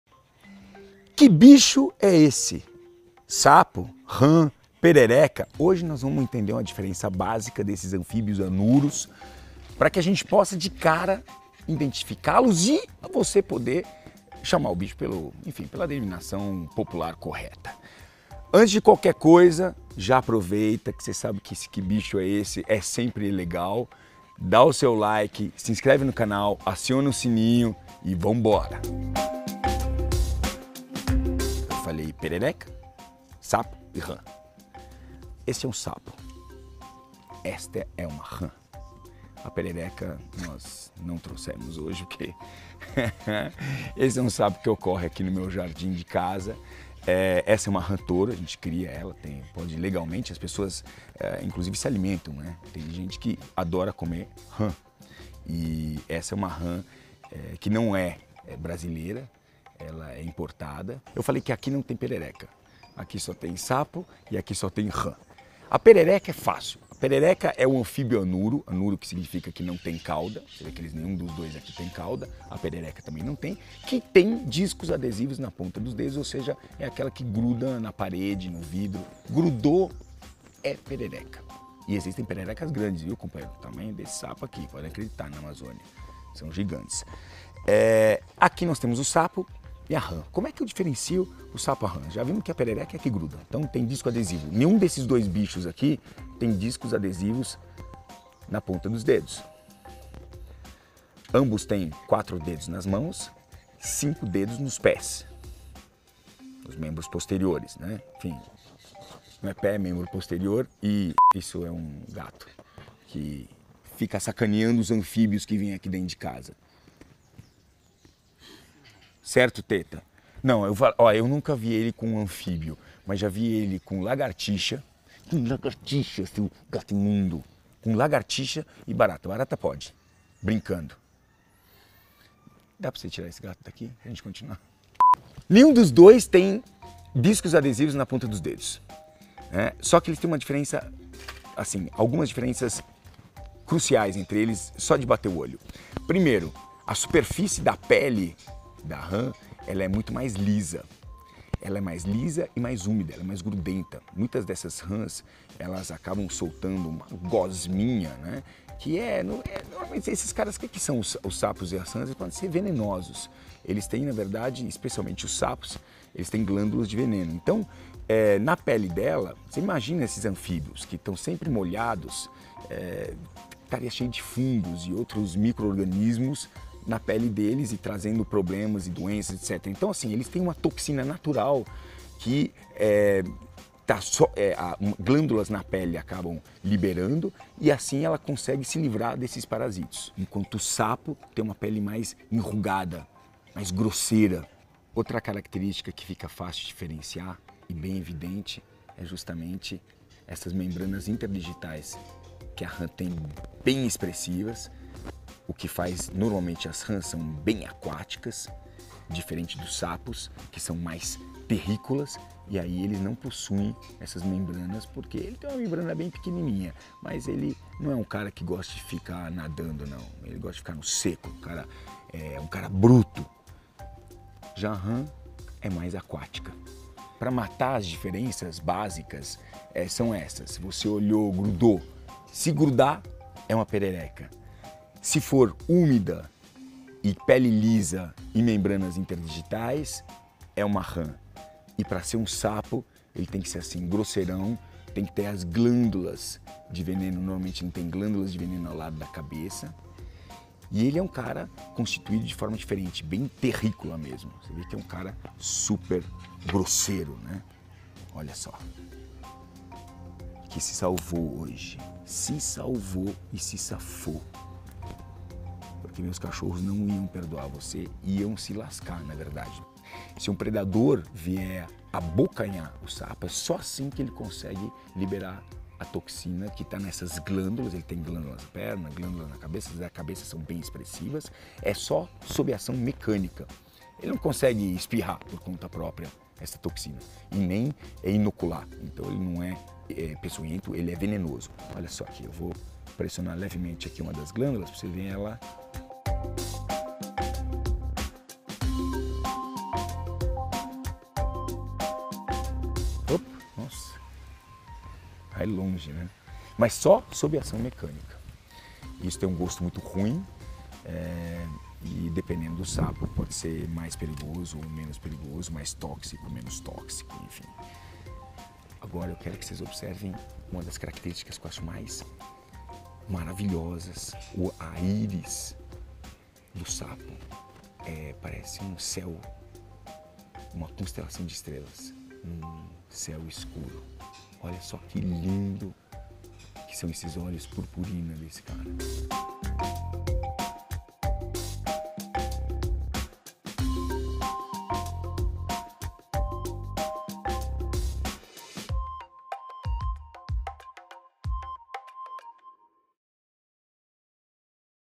SAPO OU RÃ?@Richard Rasmussen
NESSE VÍDEO VOCÊ VAI APRENDER A IDENTIFICAR CADA UM DA FORMA CORRETA